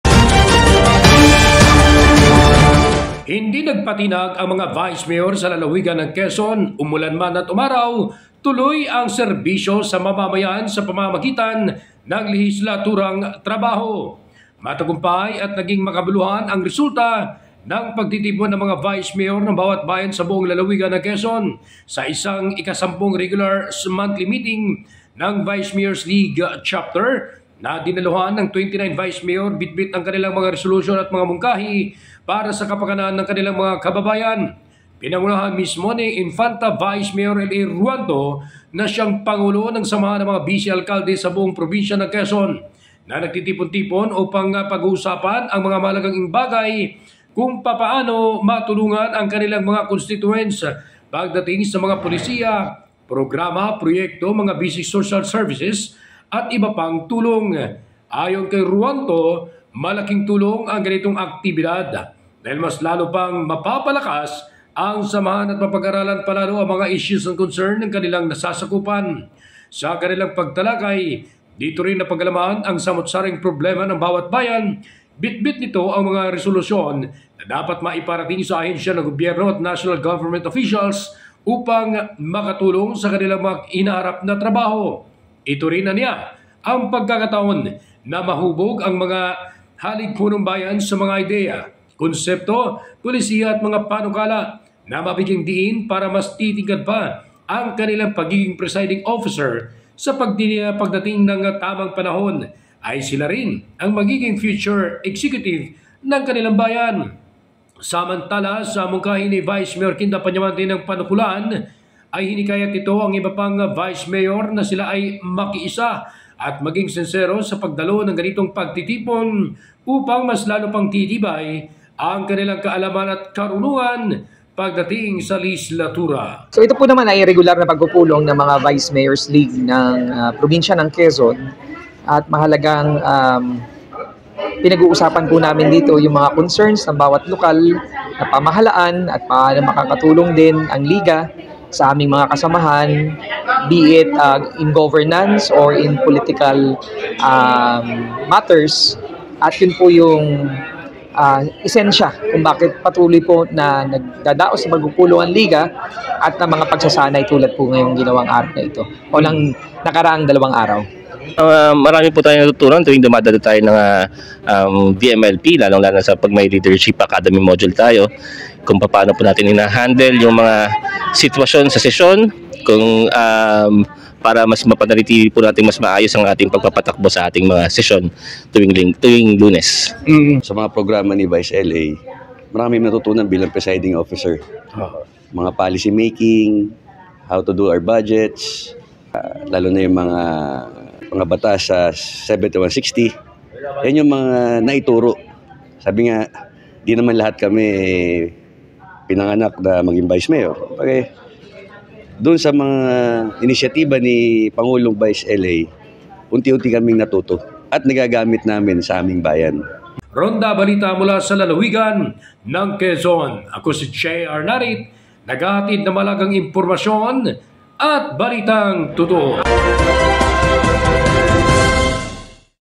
10TH REGULAR MEETING NG VMLP - QUEZON CHAPTER NAGING MAKABULUHAN AT MATAGUMPAY NA IDINAOS
PANOORIN: 10TH REGULAR MEETING NG VMLP - QUEZON CHAPTER NAGING MAKABULUHAN AT MATAGUMPAY NA IDINAOS

Narito ang Ronda Balita Probinsya ni JR NARIT